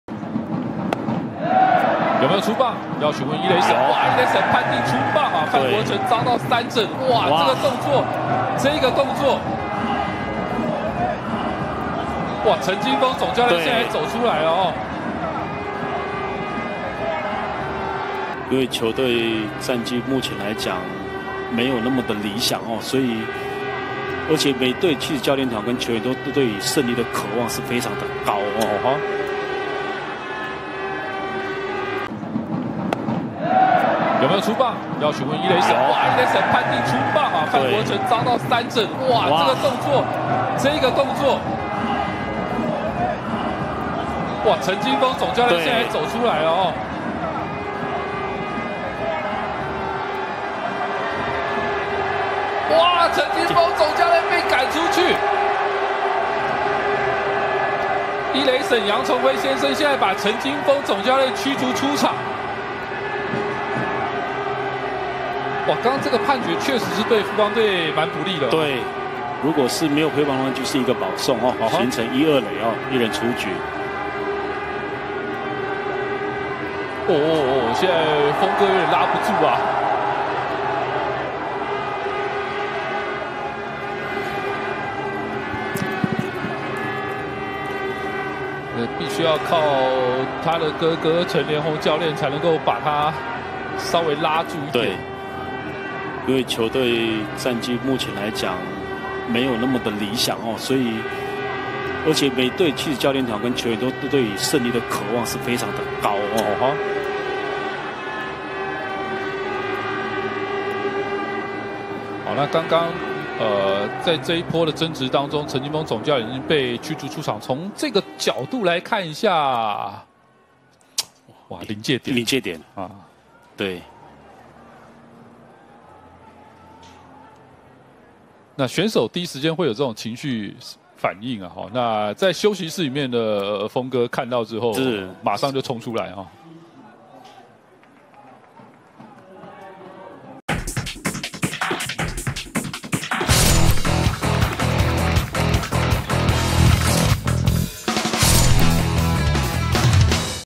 中職／范國宸遭裁定出棒吞K 富邦悍將主帥陳金鋒抗議一軍執教首遭驅逐｜三立娛樂星聞
影片來源：CPBL TV